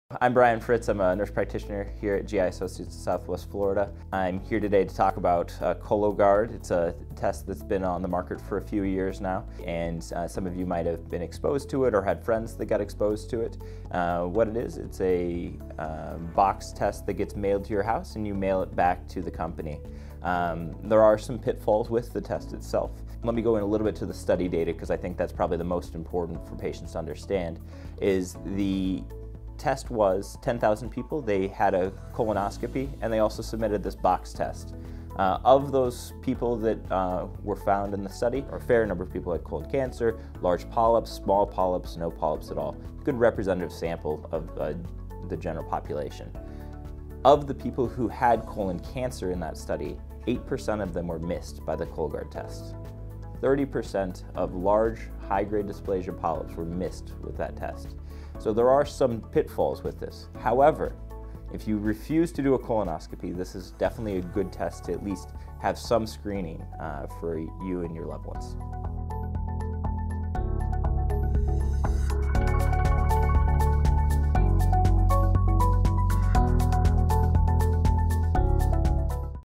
Is Cologuard a Viable Alternative to Colonoscopy? | Brian Fritz, APRN
Cologuard®, a new stool-DNA screening test for detecting colon cancer, is not as good at finding precancerous polyps as a colonoscopy, and, unlike colonoscopy, it can't remove them.

Cologuard has recently gained popularity because it is less invasive than a traditional colonoscopy test. Intrigued by lively ads with the sprightly Cologuard box jumping around on its blue legs, many adults aged 45 and older wonder if the test may work for them, allowing them to bypass a more invasive colonoscopy.

“Cologuard is a test that's been on the market for a few years now,” says Brian Fritz, APRN, an Advanced Practitioner Registered Nurse at Gastroenterology Associates of SW Florida. “Some patients might have been exposed to it or have friends that have been exposed to it. Cologuard is a box test that gets mailed to your house, you collect the sample, and mail it back to the company.”